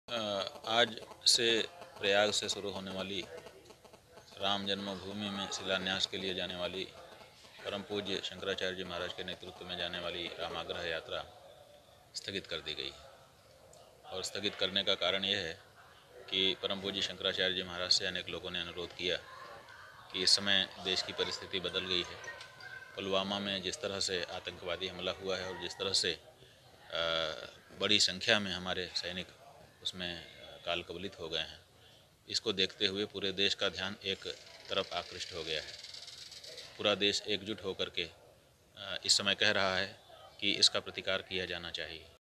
स्थगित हुई शंकराचार्य स्वामी स्वरूपानंद सरस्वती की रामाग्रह यात्रा, ये रही वजह
वाराणसी: देश में जम्मू कश्मीर के पुलवामा में हुए दर्दनाक हादसे के बाद पूरा देश गम में है. ऐसे में शंकराचार्य स्वामी स्वरूपानंद सरस्वती ने भी राम मंदिर निर्माण को लेकर रामाग्रह यात्रा को स्थगित कर दिया. उन्होंने कहा कि वह भी देश के साथ हैं. ऐसे में नई तारीख की घोषणा भी काशी से हो सकती है.